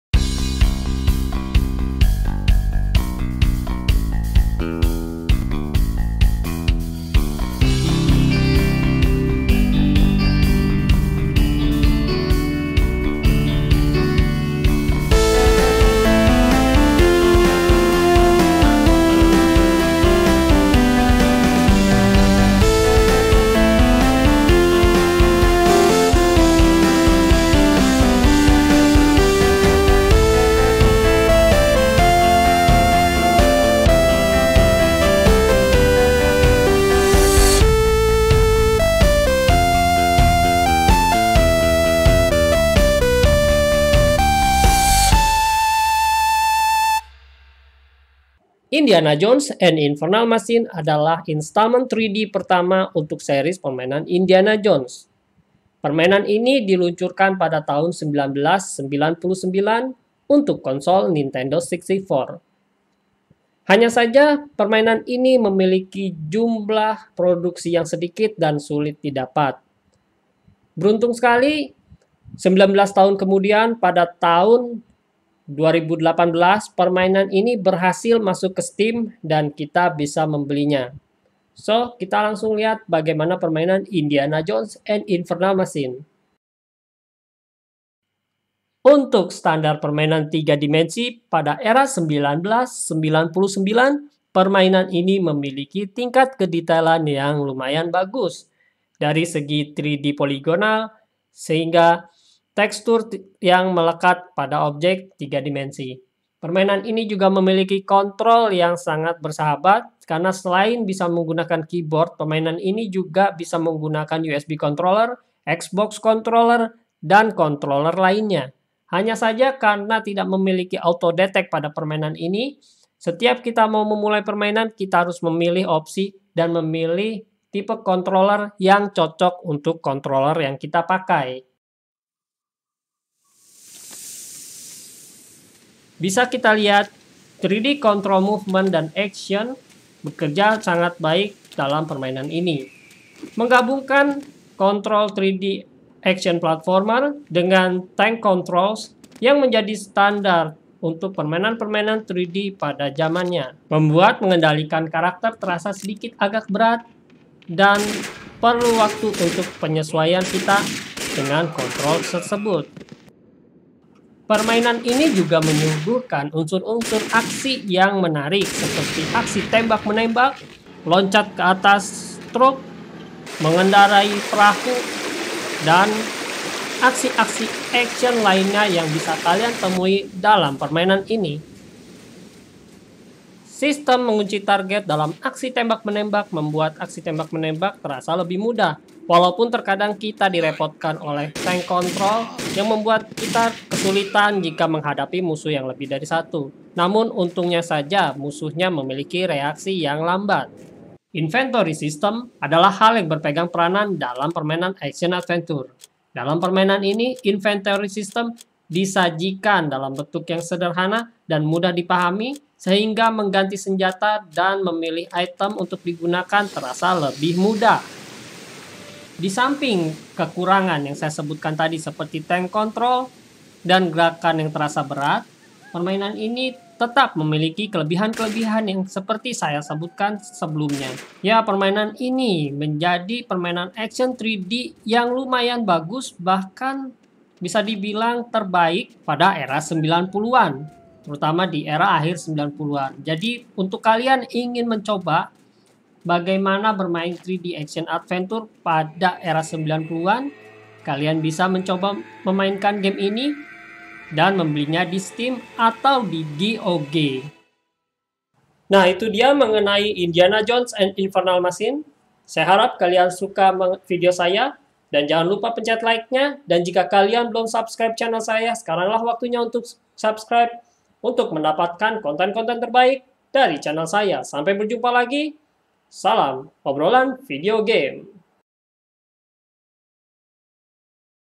Obrolan Video Game episode 10 : Indiana Jones and Infernal machine
Lanjut lagi di Obrolan video game, kali ini kita membahas permainan yang pernah di rilis sekitar 20 tahun lalu.. Indiana Jones and Infernal machine...